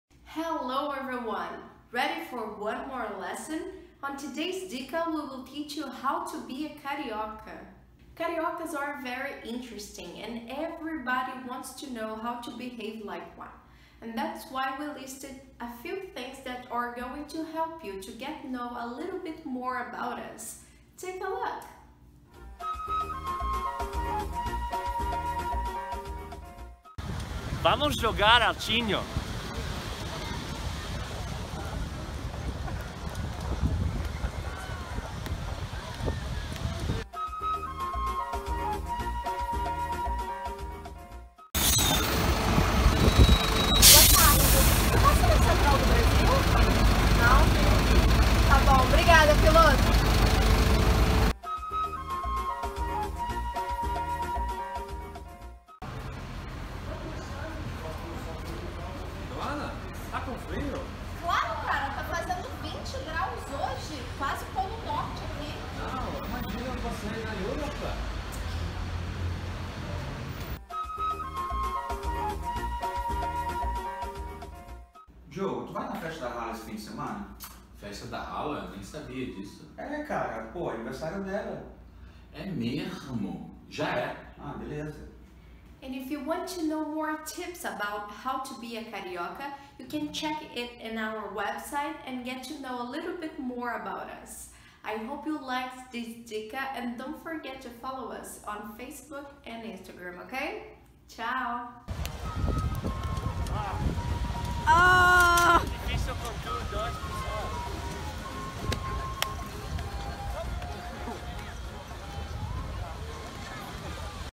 How to be a Carioca - A Dica do Dia
Hi guys! Today we will show you how to be a Carioca with four tips!

If you have doubts about Portuguese talk to us on our Twitter